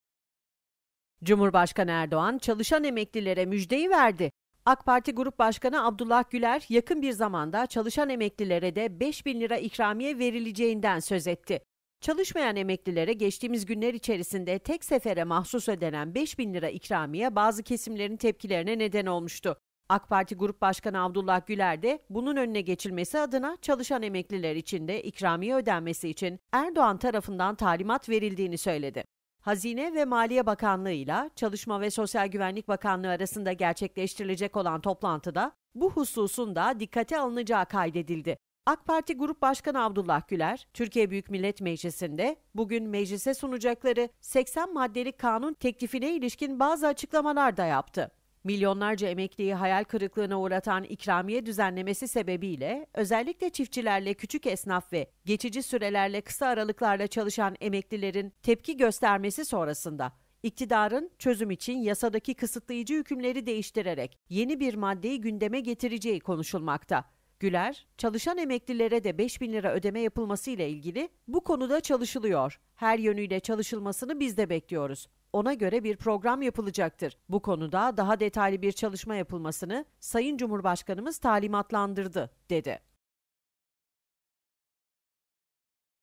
Cumhurbaşkanı Erdoğan Talimatı Verdi | Çalışan Emeklilere 5.000 TL İkramiye Geliyor
Cumhurbaşkanı Erdoğan çalışan emeklilere müjdeyi verdi. AK Parti Grup Başkanı Abdullah Güler yakın bir zamanda çalışan emeklilere de 5.000 TL ikramiye verileceğinden söz etti.
Çalışmayan emeklilere geçtiğimiz günler içerisinde tek sefere mahsus ödenen 5.000 TL ikramiye bazı kesimlerin tepkilerine neden olmuştu. AK Parti Grup Başkanı Abdullah Güler’de bunun önüne geçilmesi adına çalışan emekliler içinde ikramiye ödenmesi için Erdoğan tarafından talimat verildiğini söyledi.

Çalışma Hazine ve Maliye Bakanlığı ile Çalışma ve Sosyal Güvenlik Bakanlığı arasında gerçekleştirilecek olan toplantıda bu hususunda dikkate alınacağı kaydedildi. AK Parti Grup Başkanı Abdullah Güler, TBMM’de bugün Meclis’e sunacakları 80 maddelik kanun teklifine ilişkin bazı açıklamalarda yaptı.

Milyonlarca emekliyi hayal kırıklığına uğratan ikramiye düzenlemesi sebebiyle özellikle çiftçilerle küçük esnaf ve geçici sürelerle kısa aralıklarla çalışan emeklilerin tepki göstermesi sonrasında iktidarın çözüm için yasadaki kısıtlayıcı hükümleri değiştirerek yeni bir maddeyi gündeme getireceği konuşulmakta.

Güler, “(Çalışan emeklilere de 5 bin lira ödeme yapılması) Bu konuda çalışılıyor. Her yönüyle çalışılmasını biz de bekliyoruz. Ona göre bir program yapılacaktır. Bu konuda daha detaylı bir çalışma yapılmasını Sayın Cumhurbaşkanımız talimatlandırdı.” dedi.